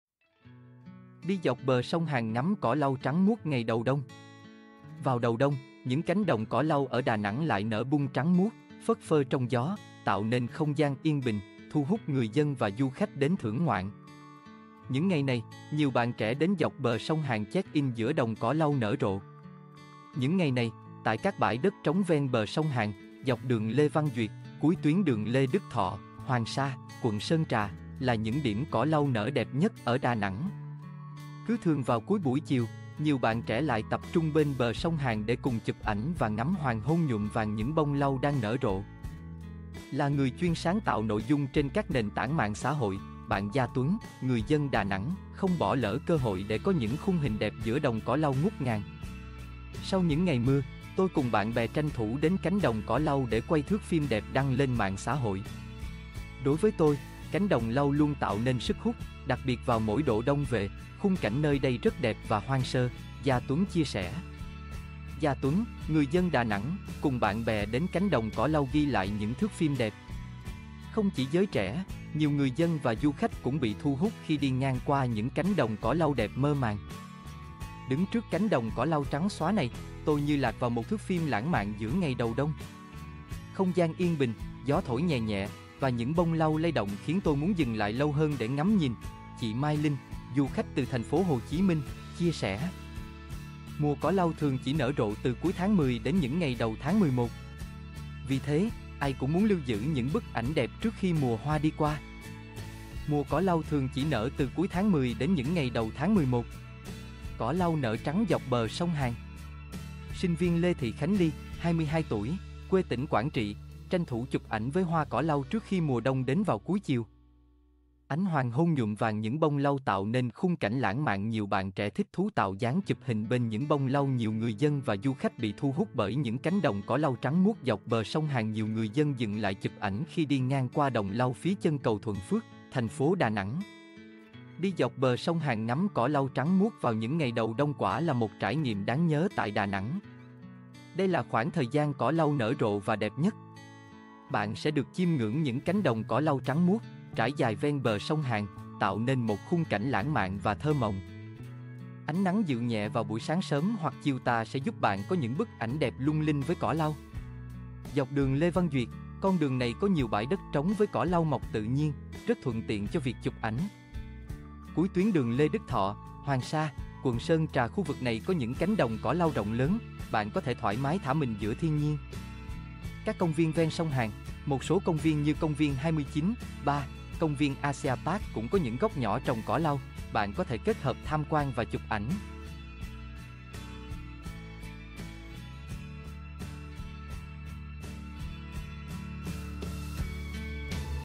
Đi dọc bờ sông Hàn ngắm cỏ lau trắng muốt ngày đầu đông
Vào đầu đông, những cánh đồng cỏ lau ở Đà Nẵng lại nở bung trắng muốt, phất phơ trong gió, tạo nên không gian yên bình, thu hút người dân và du khách đến thưởng ngoạn.

Những ngày này, nhiều bạn trẻ đến dọc bờ sông Hàn check-in giữa đồng cỏ lau nở rộ

Những ngày này, tại các bãi đất trống ven bờ sông Hàn, dọc đường Lê Văn Duyệt, cuối tuyến đường Lê Đức Thọ - Hoàng Sa (quận Sơn Trà)… là những điểm cỏ lau nở đẹp nhất ở Đà Nẵng. Cứ thường vào cuối buổi chiều, nhiều bạn trẻ lại tập trung bên bờ sông Hàn để cùng chụp ảnh và ngắm hoàng hôn nhuộm vàng những bông lau đang nở rộ. Là người chuyên sáng tạo nội dung trên các nền tảng mạng xã hội, bạn Gia Tuấn (người dân Đà Nẵng) không bỏ lỡ cơ hội để có những khung hình đẹp giữa đồng cỏ lau ngút ngàn. "Sau những ngày mưa, tôi cùng bạn bè tranh thủ đến cánh đồng cỏ lau để quay thước phim đẹp đăng lên mạng xã hội. Đối với tôi, cánh đồng lau luôn tạo nên sức hút, đặc biệt vào mỗi độ đông về, khung cảnh nơi đây rất đẹp và hoang sơ", Gia Tuấn chia sẻ.